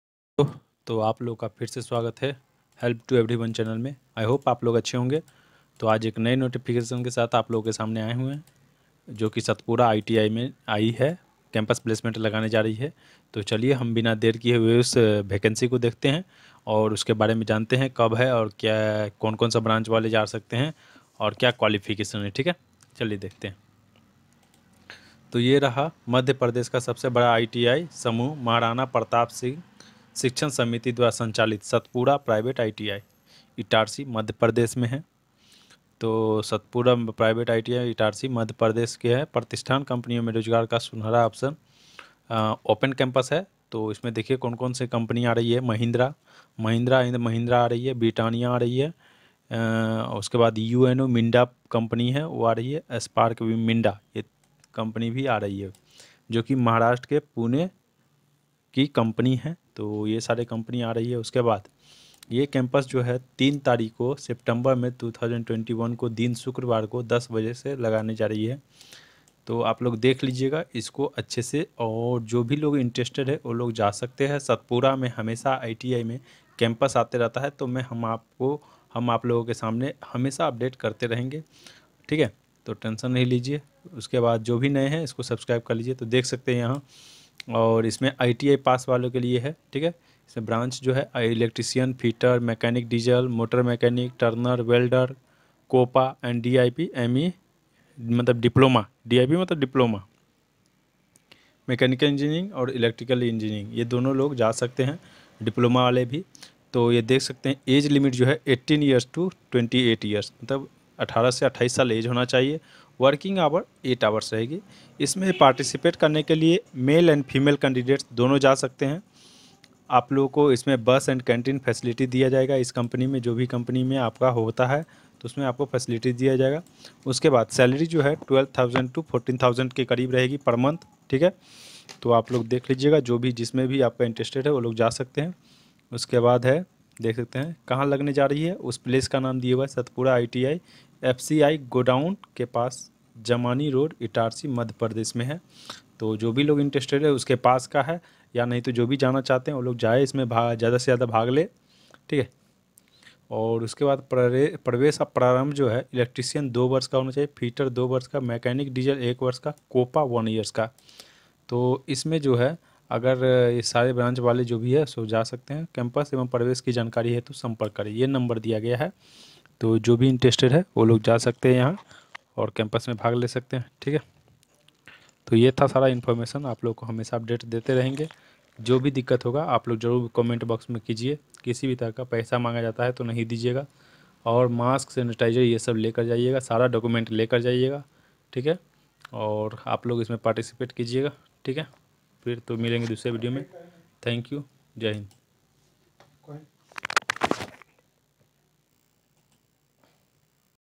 Open Campus Placement Drive for Satpura ITI College job Campus Placement 2021
Details:- Company Name

1. Mahindra and Mahindra Pvt Ltd

2. Britannia Indian Pvt Ltd

3. UNO Minda Pvt Ltd

4. Spark Minda Pvt Ltd

How to Open Campus Placement 2021

Qualification

ITI Pass Out Trade

Electrician
Fitter
Machinist
Diesel Mechanical
Turner
Welder
Copa
Dip. , ME,EE

Salary and Benefits

Salary Rs 12,000/- ₹ to 14,500/- ₹ per month

Working Hrs. 8 Hrs.

Facilities - Canteen and Bus facility

Gender - Male and Female Candidate Apply

Campus Placement View

Satpuda ITI near FCI, Jamani Road, Itarsi Madhya Pradesh

Campus Placement Date

Date 3 September 2021 time 10 a.m.

Notes

कोविड-19 के चलते हुए सरकार द्वारा दिए गए दिए गए निर्देशों का पालन करना है
 मास्क, सोशल डिस्टेंसिंग, सेनीटाइजर, पानी की बोतल साथ में मैं अवश्य लेकर आएं